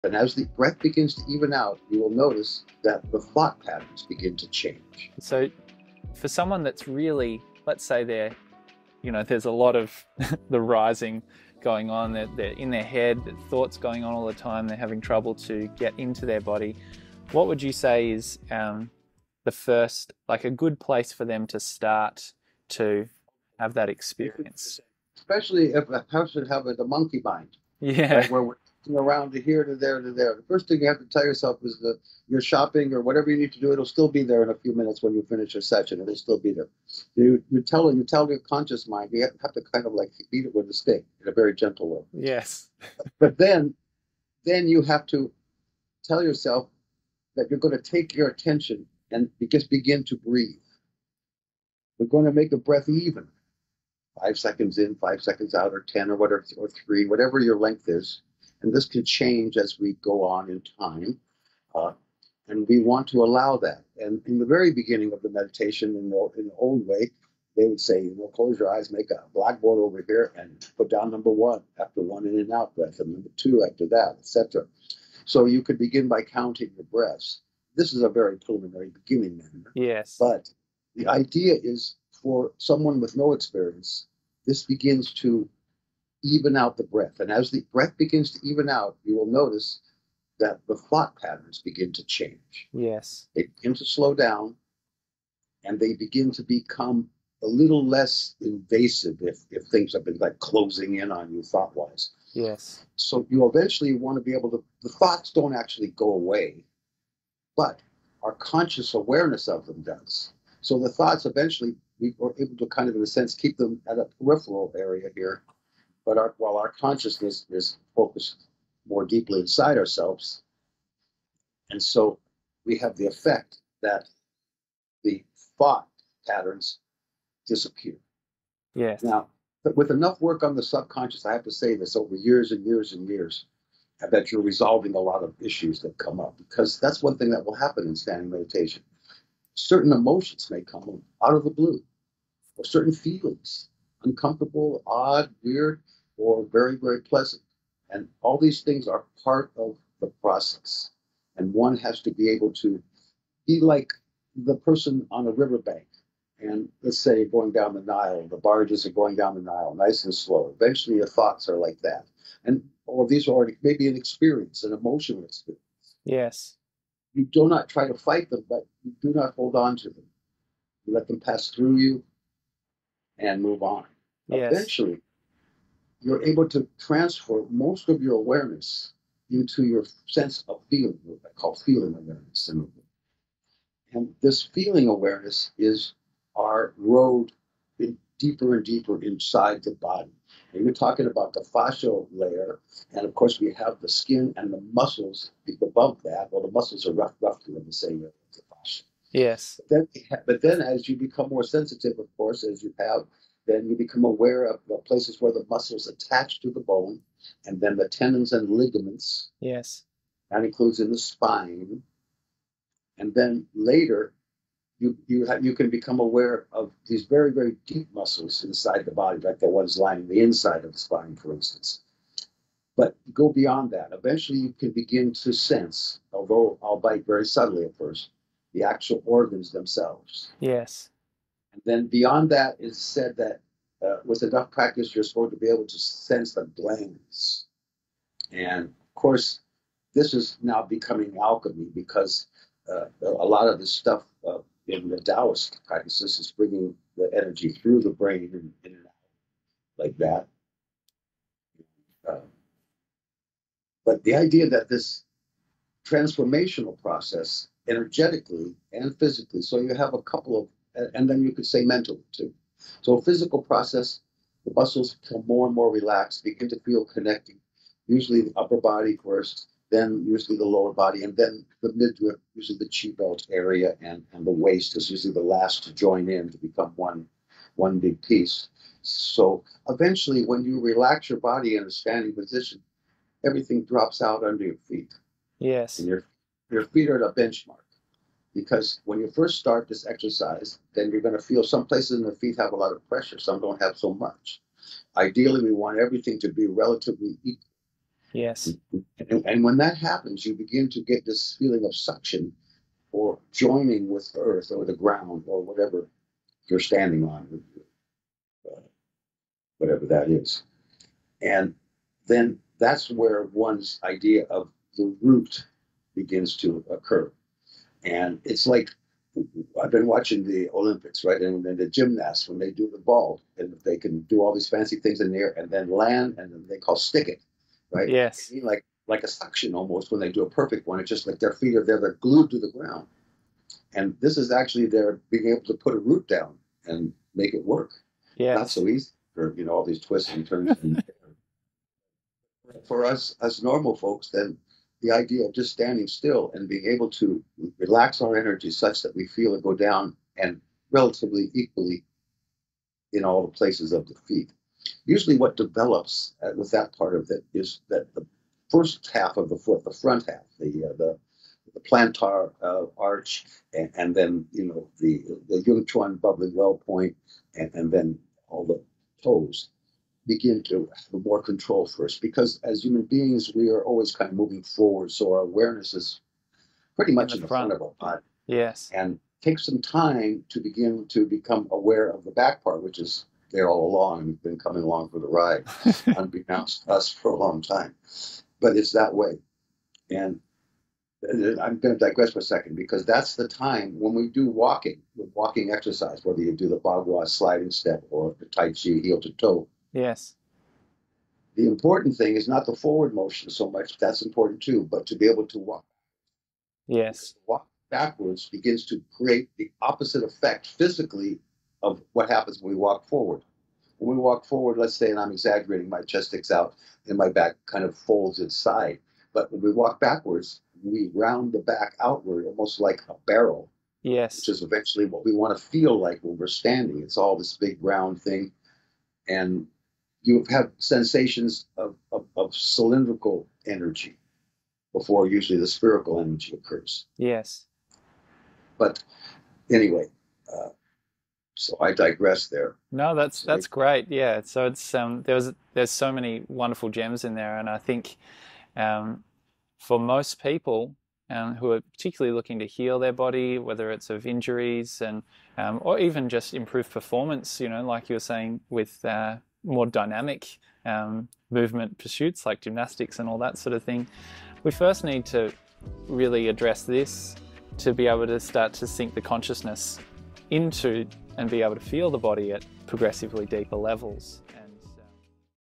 Get grounded (out of your head) with standing meditation: Mark Cohen | Inside Zhan Zhuang
Learn standing meditation to heal your body and expand your consciousness